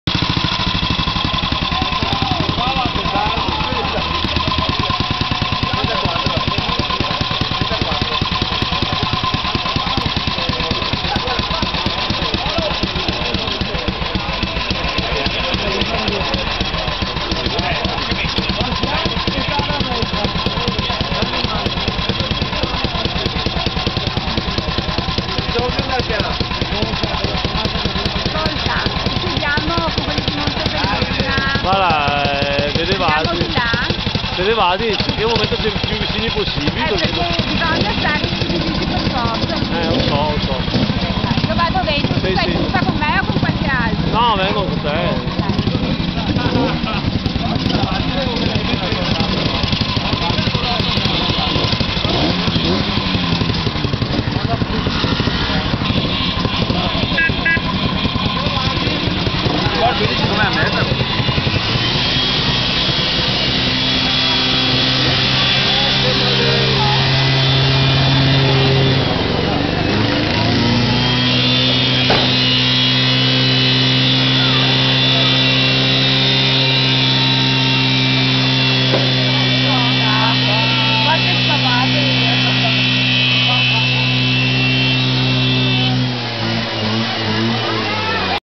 Motori rombanti , GUZZI del 34' per la festa dei SIV
Onore al motore di una guzzi alla festa del vespa club sgallinatinvespa di sabato 11 ottobre all'osteria "da coppi"